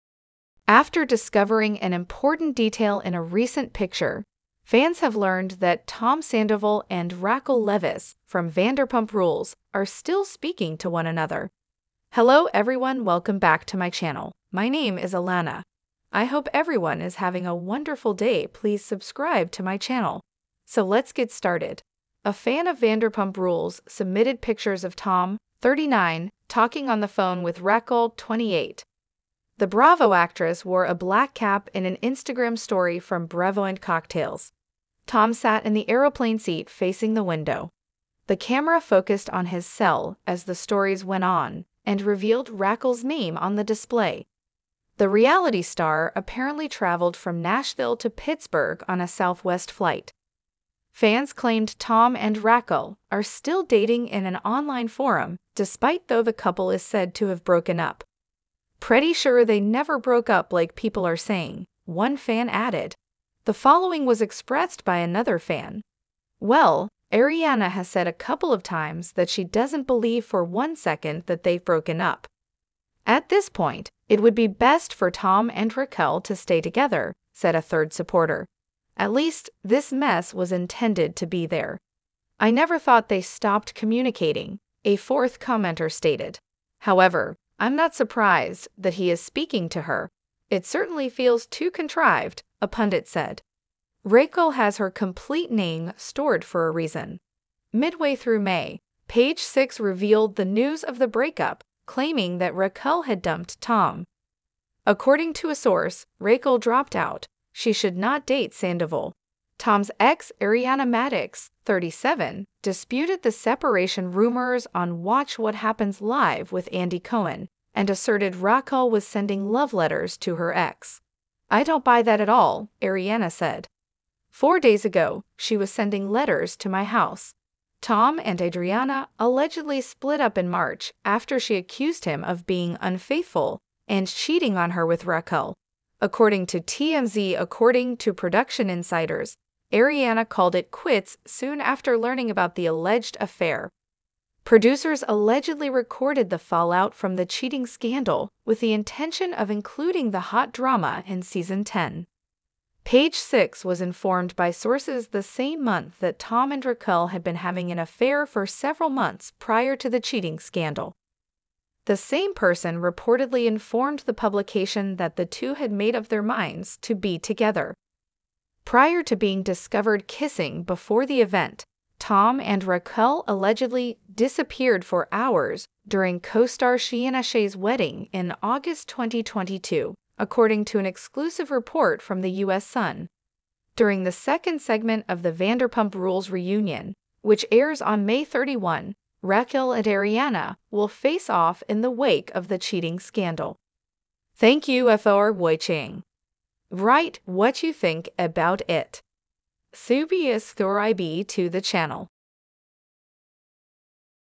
Fans Call Out Tom Sandoval: Still Talking to Raquel Leviss? Zooming
Stay Tune With CELEBRITY GOSSIPY
Thank For Your Support "Vanderpump Rules Fans Call Out Tom Sandoval: Still Talking to Raquel Leviss? Zooming in on His Phone in New Pic!"

Raquel Leviss
Reality TV
Tom Sandoval
Vanderpump Rules